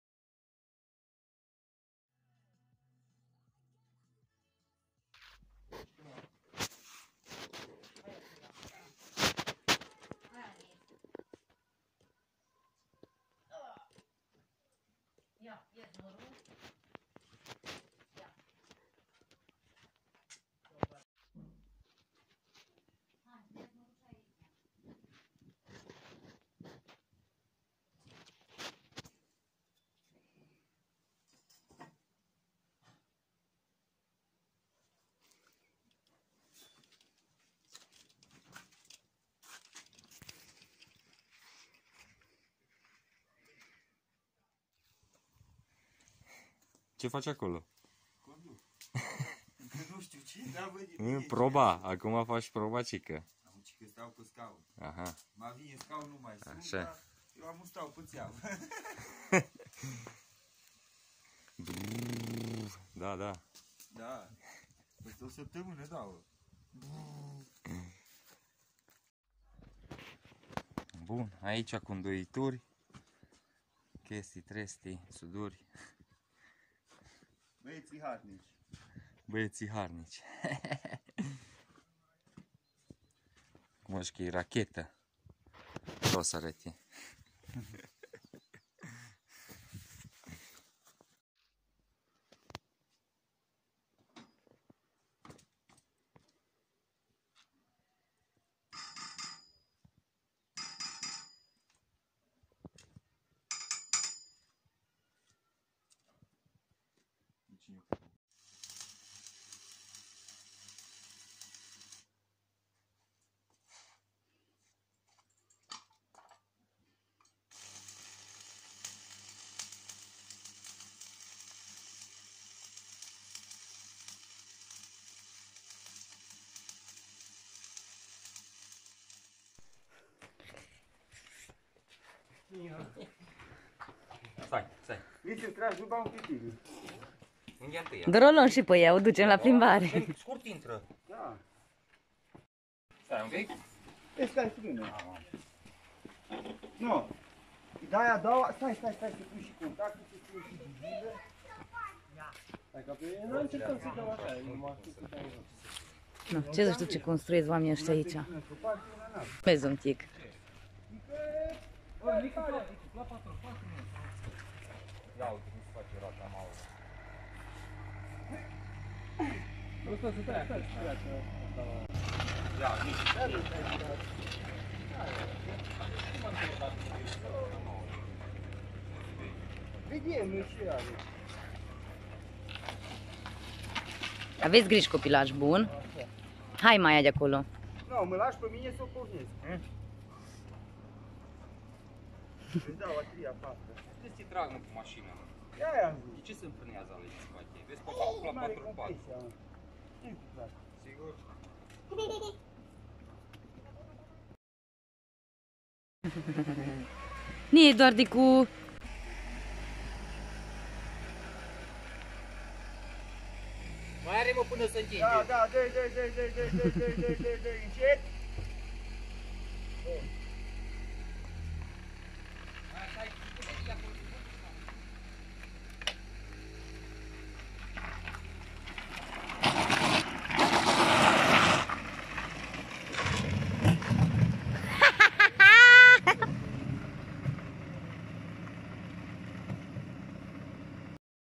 Buggy 4x4 cu motor de dacia papuc facut acasa
am adunat cateva filmulete cu constructia "Tractorasului"
o sa mai postez si cu el in actiune